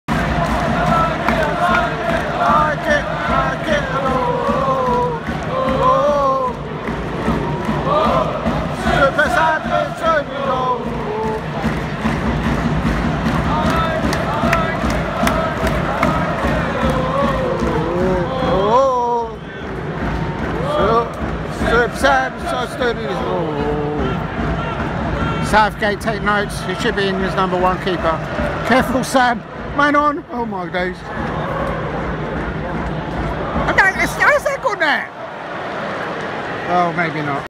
Crystal Palace 0-0 Nottingham Forest (FT) 45'+2' Sam Johnstone song from the Palace Ultras 🦅
Saturday 07 October 2027 kickoff 17:30.

Crystal Palace
STARTING LINEUP
Sam Johnstone, Tyrick Mitchell, Joachim Andersen, Marc Guéhi, Nathaniel Clyne, Jeffrey Schlupp, Jairo Riedewald, Will Hughes, Jean-Philippe Mateta, Jordan Ayew & Odsonne Édouard.
SUBSTITUTES
Rob Holding, James Tomkins, Malcolm Ebiowei, Chris Richards, Remi Matthews, Jesurun Rak-Sakyi, David Ozoh, Ademola Ola-Adebomi & Jadan Raymond.

Nottingham Forest
STARTING LINEUP:
Matt Turner, Sèrge Aurier, Murillo, Willy-Arnaud Boly, Harry Toffolo, Ibrahim Sangaré, Nicolás Domínguez, Orel Mangala, Callum Hudson-Odoi, Morgan Gibbs-White & Chris Wood.
SUBSTITUTES
Joe Worrall, Neco Williams, Cheikhou Kouyaté, Andrey Santos, Anthony Elanga, Ryan Yates, Odysseas Vlachodimos, Divock Origi & Gonzalo Montiel.